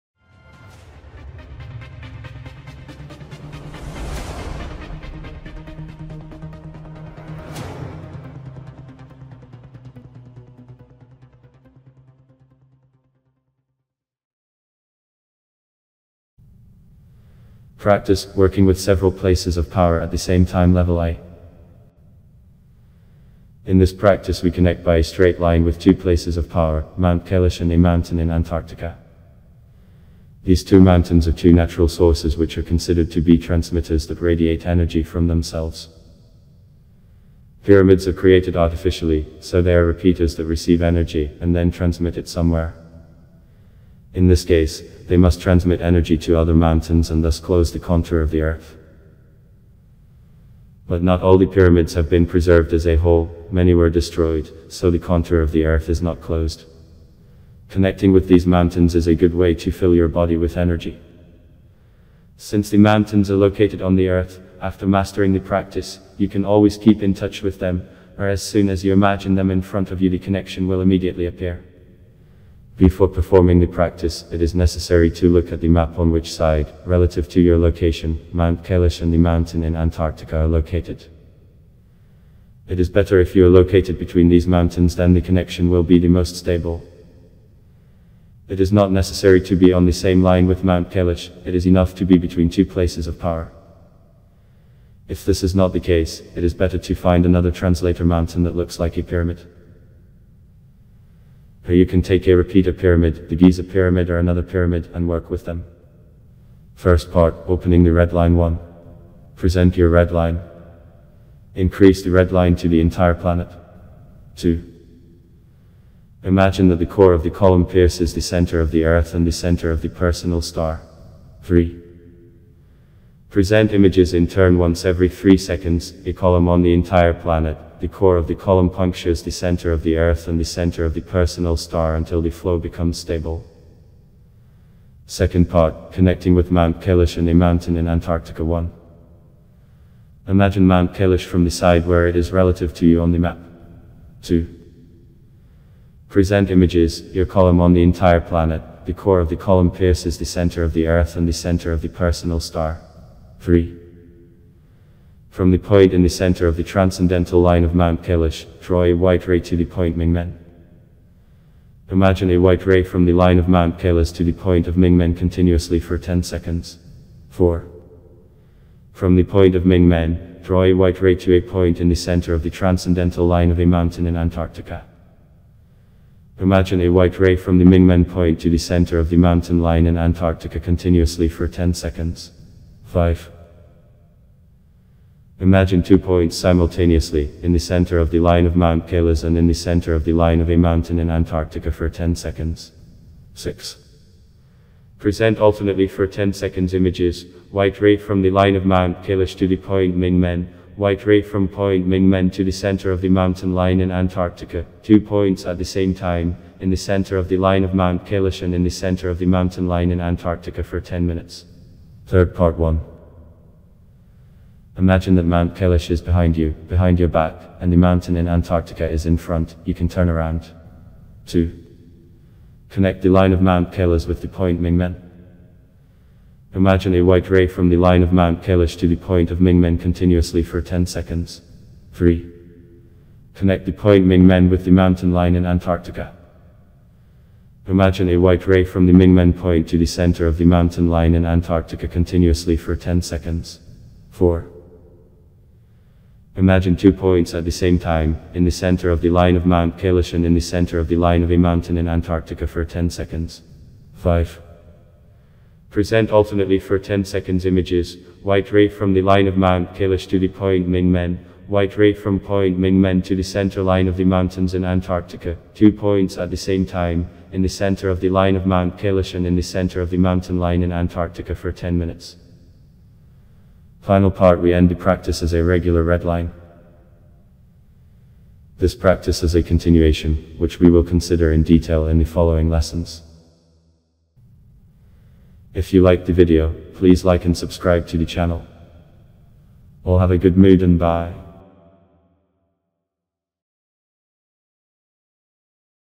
Practice "Working with several places of power at the same time level II"
Practice
 "Working with several places of power at the same time level II"

Excerpt from the book "The science of the internal force or increase of the human magnetic field" part 1
author Svetlana Vezhnovets
illustrator Alexandra Sleight

Ba Duan Jin - buy a book as an application for android